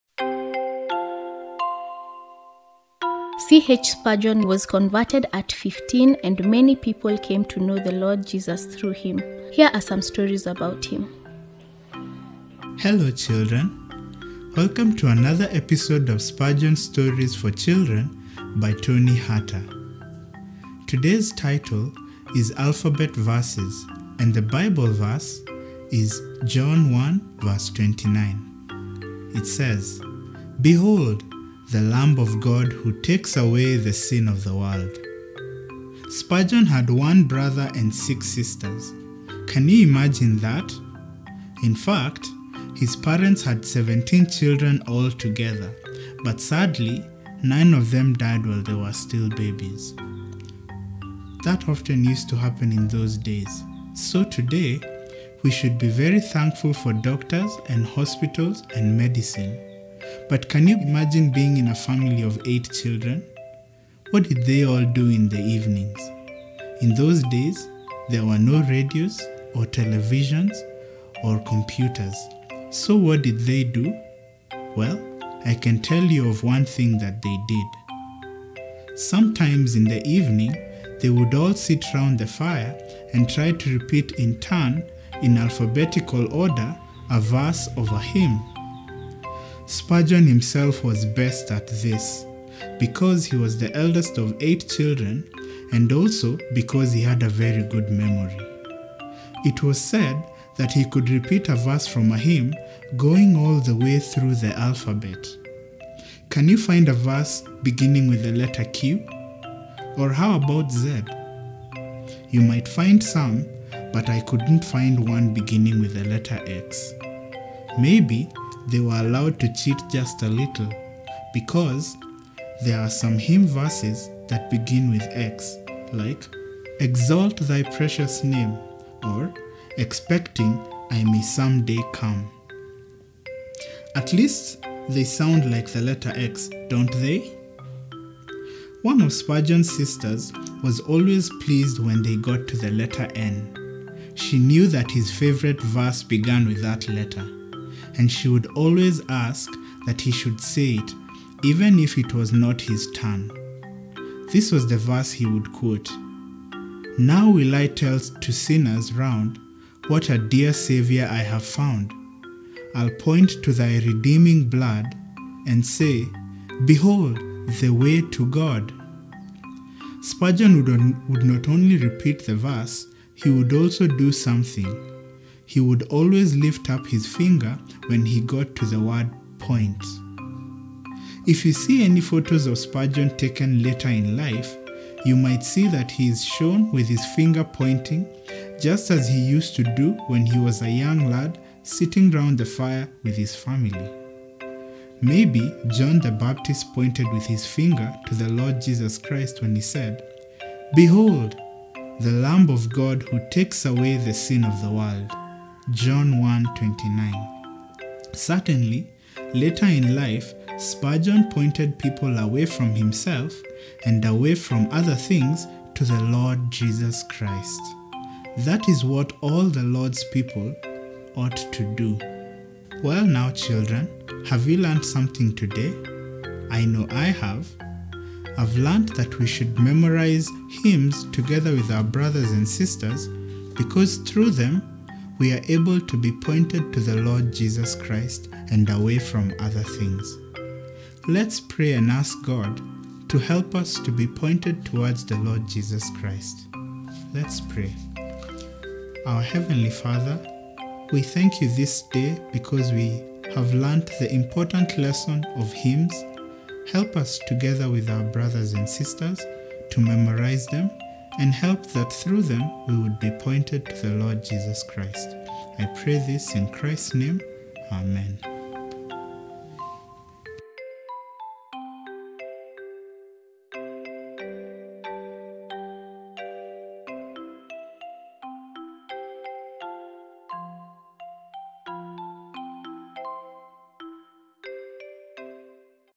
Spurgeon Stories for Children (Book 1): Chapter 13 : Alphabet Verses
This book began life as a series of children's talks based on the life of C.H. Spurgeon, each one meant to teach a truth of Scripture to the very young. Spurgeon had a great love for children.

He founded an orphanage and regularly visited the children there. They would throng around him and he knew virtually all of them by name. On one occasion, when Spurgeon was in Menton in France, a boy in the orphanage died. Spurgeon in response wrote to all the boys in the orphanage: "I cannot bear to think of one boy going from the orphanage to hell; that would be terrible indeed! I often think about you all. I want to see you all happy here and hereafter."

This book is written with the hope and prayer that children may be led by the Lord to find salvation in Christ, and learn truths from the Bible that will help them to live for His glory. Great read-aloud stories for children 12 and under.